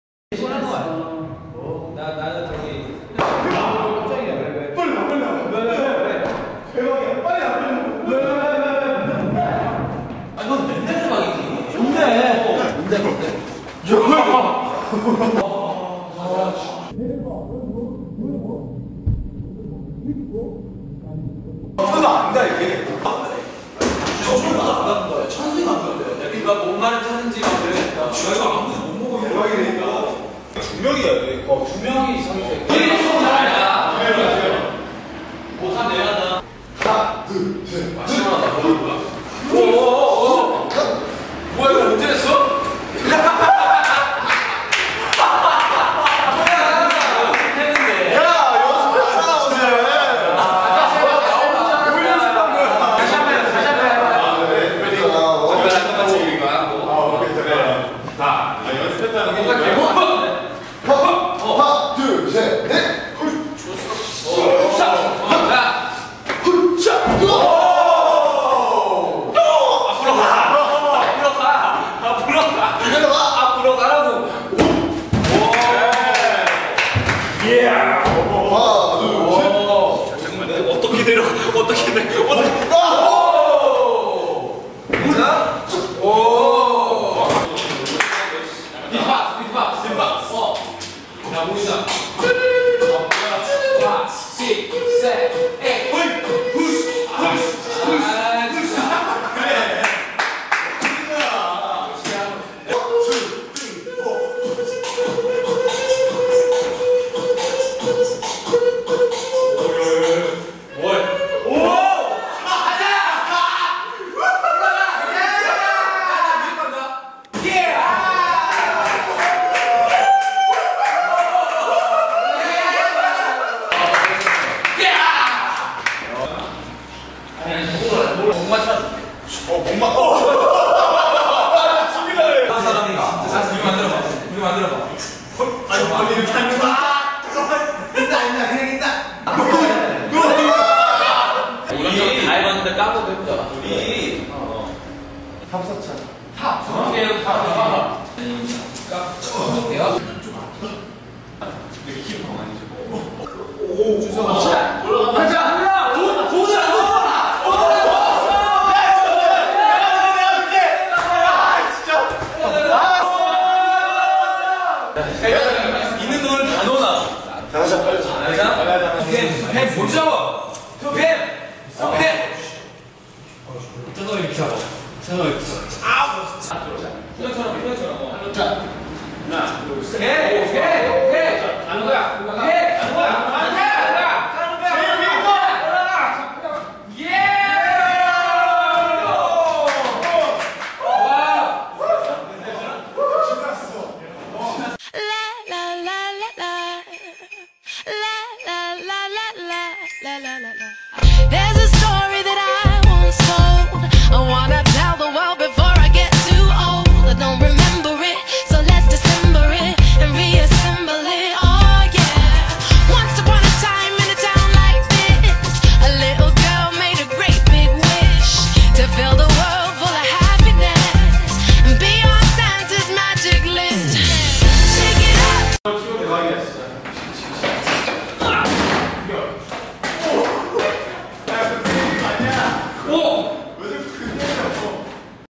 [Vietsub - 2ST] Cocacola Event - 2PM Hidden Camera Vending Machine
Vtrans/Time by Chibi
Edit/Enco by Meo
Upload by UpTeam